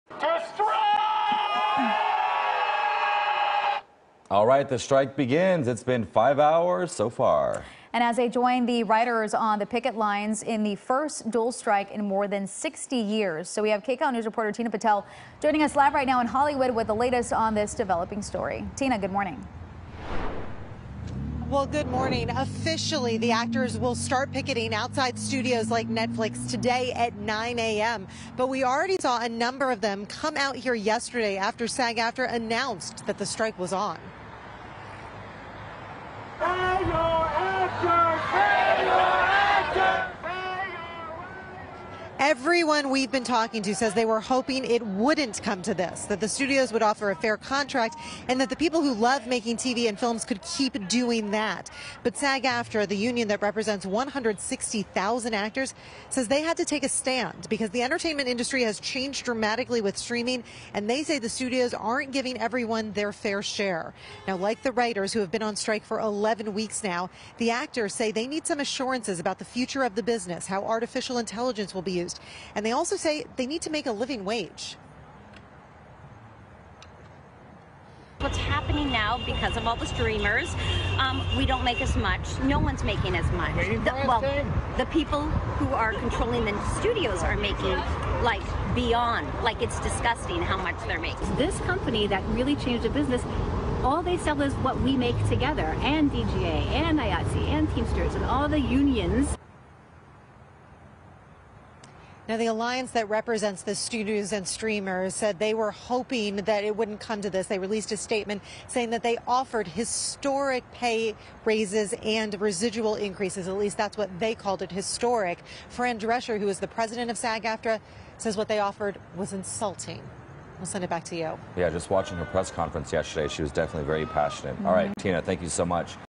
Actors' strike begins
Members of the SAG-AFTRA actors union will begin picketing outside L.A.-area studios today, one day after the union's National
Board voted unanimously to issue a strike order -- leading union President Fran Drescher to angrily denounce the major studios as "a very greedy entity" while the studios' bargaining arm countered that it had offered "historic" benefit boosts.